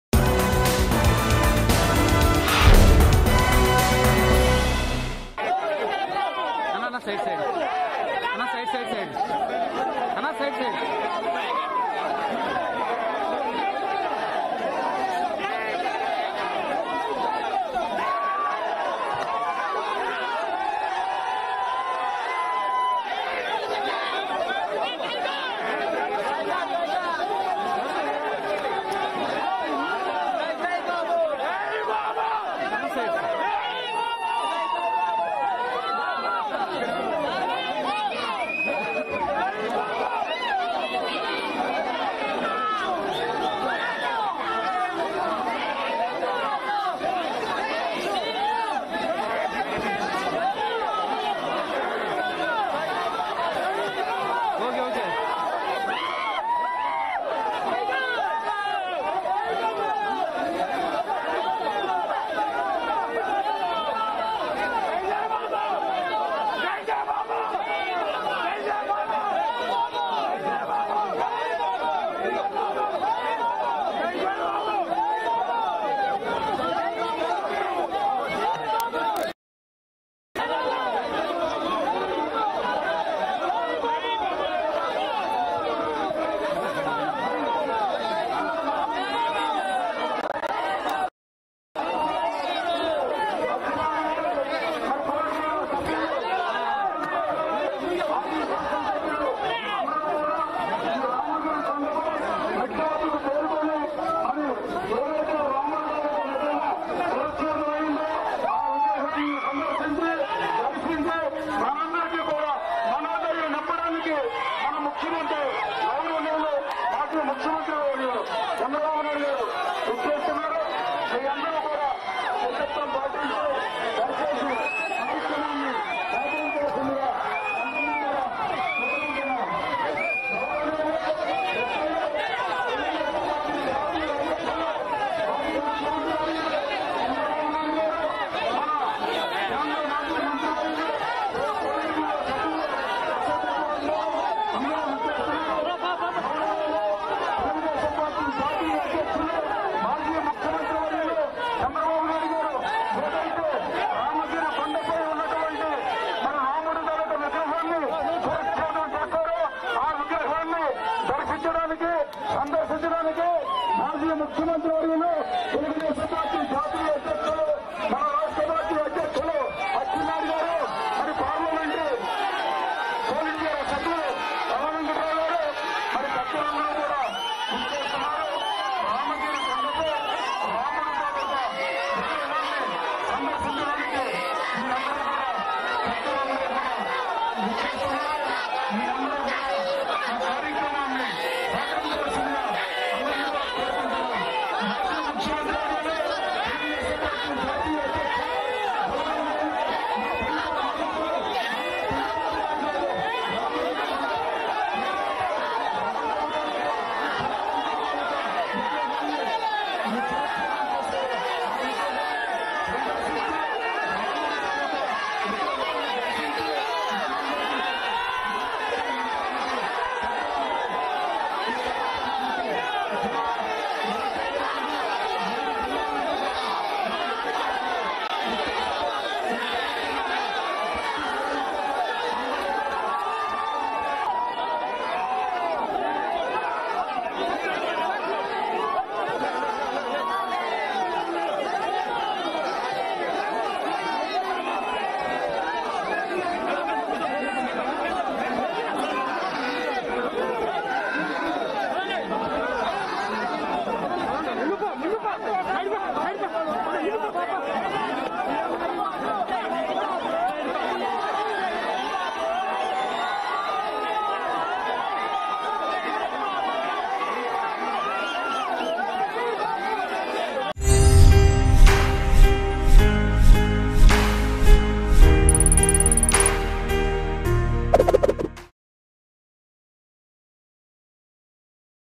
TDP Chief Chandrababu Naidu Reaches Ramatheertham Temple In Vizianagaram | AP News | Mango News
TDP Chief Chandrababu Naidu Reaches Ramatheertham Temple In Vizianagaram.